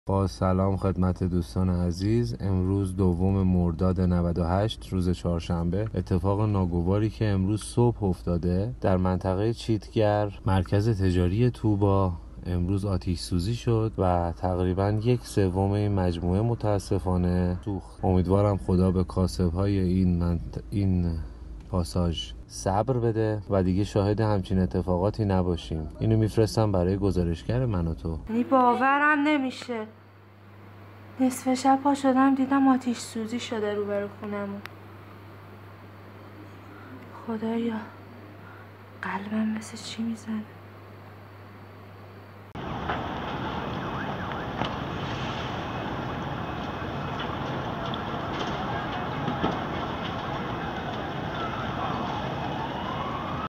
آتش سوزی در مجتمع تجاری طوبی در محدوده چیتگر - تهران
به گفته ملکی این حادثه تا کنون مصدوم و تلفات جانی نداشته.